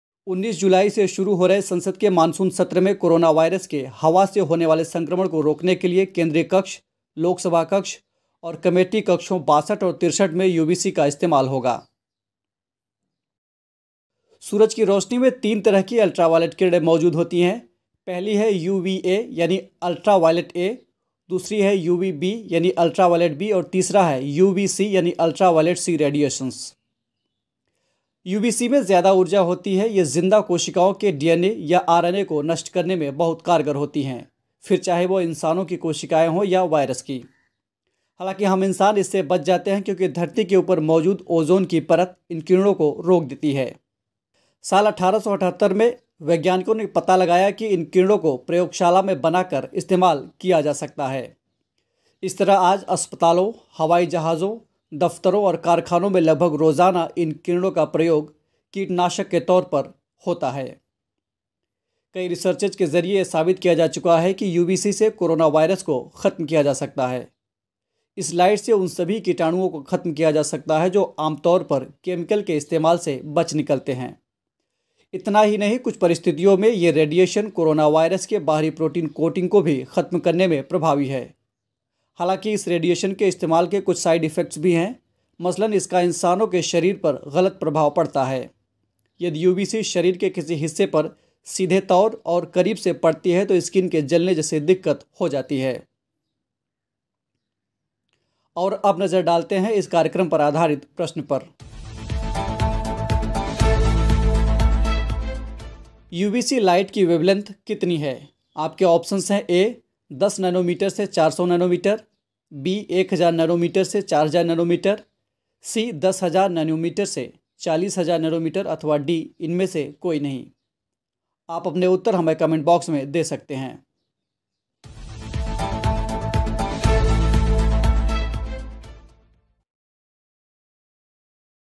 क्या है UV-Cजिसका इस्तेमाल संसद मे कोरोना वायरस के खात्मे के लिए किया जायेगा?
By-VIKASH SINGH PARIHAR